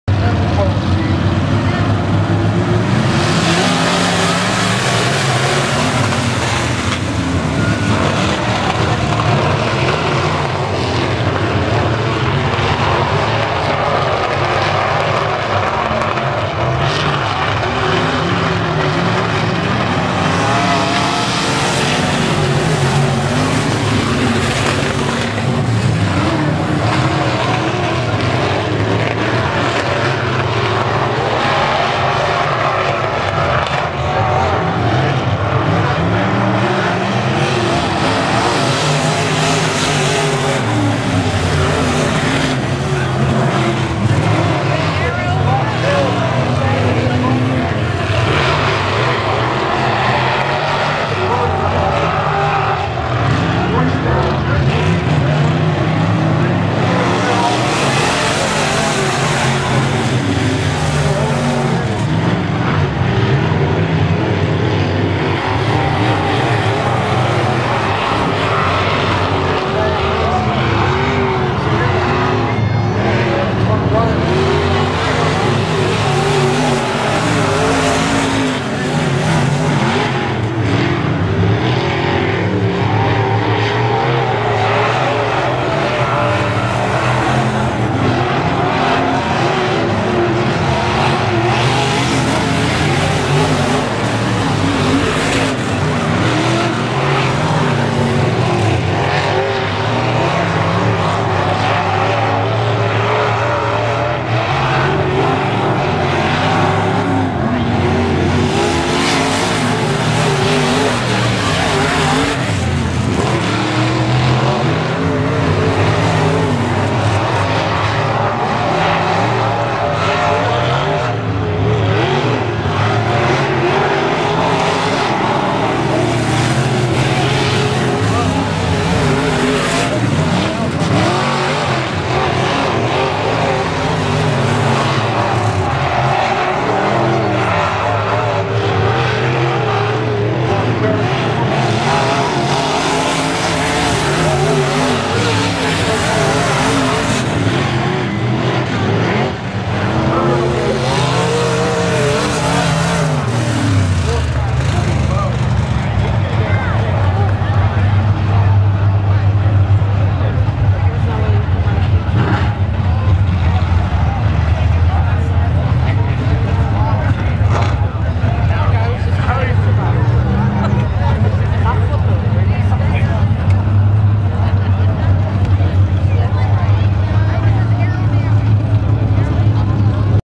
Hummingbird Speedway (5-27-23): Andy Man's Car Care Economod Heat Race #2
Heat Race Results:

1st) #0 Dennis Asel
2nd) 57S Royce Stanley Jr.
3rd) 12C Chip Davis
4th) 21L Dave Lowe
5th) 21T Orin Taylor